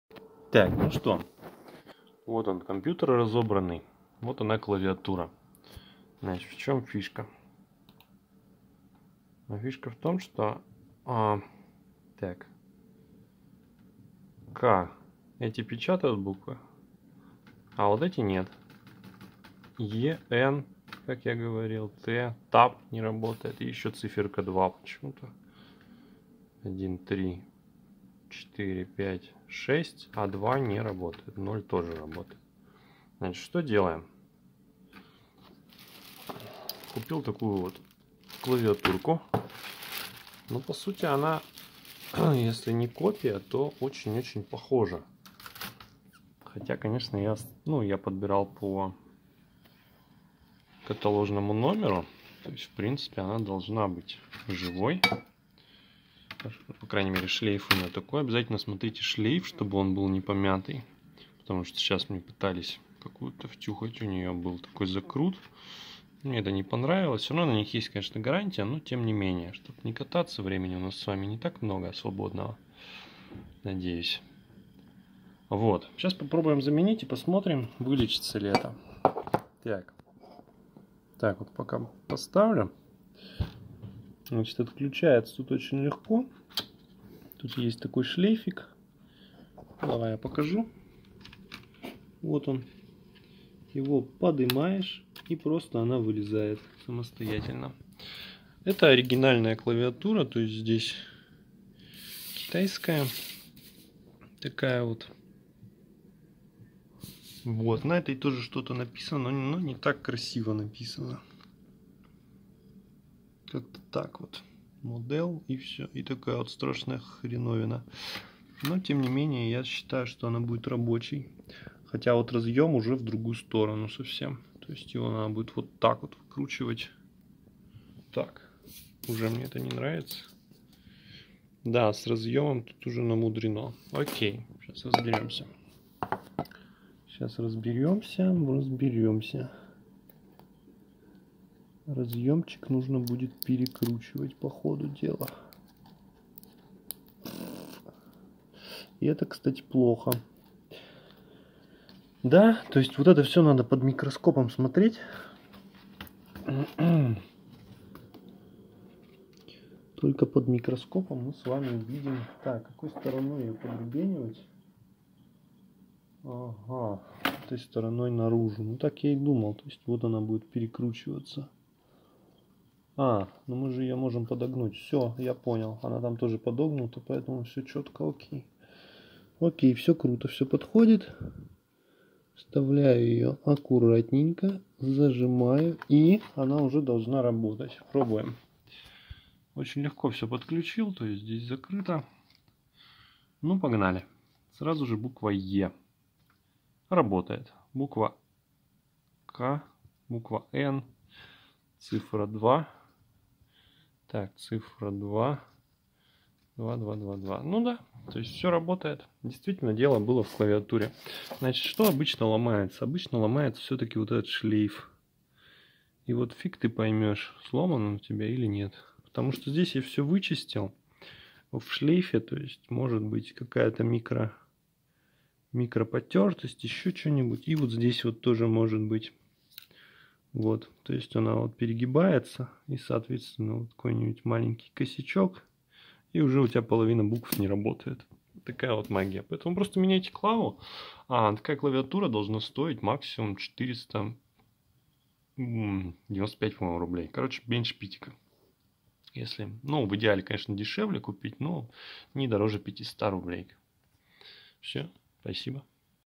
Поменять клавиатуру на ноутбуке asus k540
Показываю как я поменял клавиатуру на своём ноутбуке асус k540lj самостоятельно